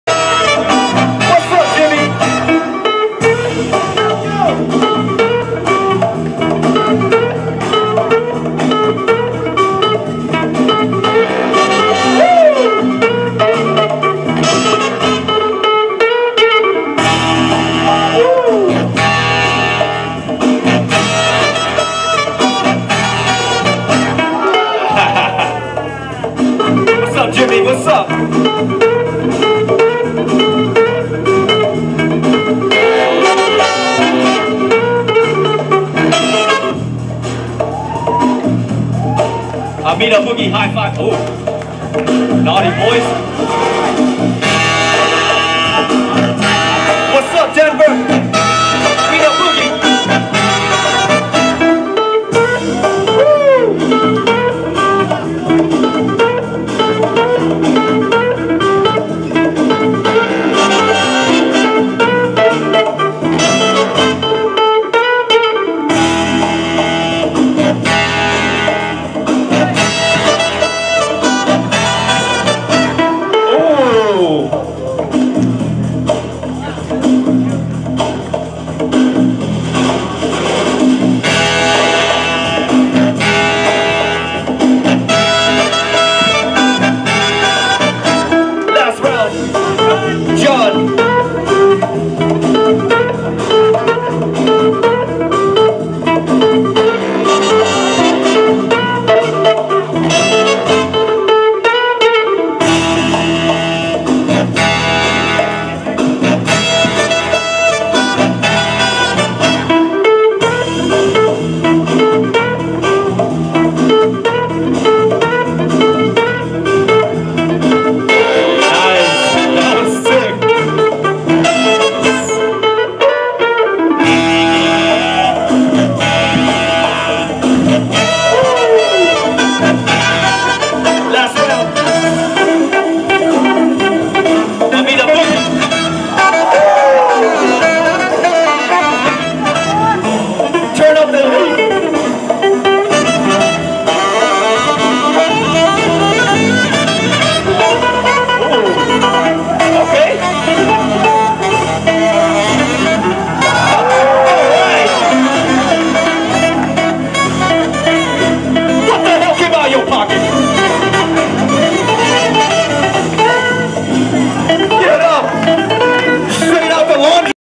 Calamities 12th: (Prelims) #19
(Prelims) 2v2 B-Boy Battles at Calamities 12th Anniversary Jam in San Diego, CA 6/14/14. Comment who is in this and I will update!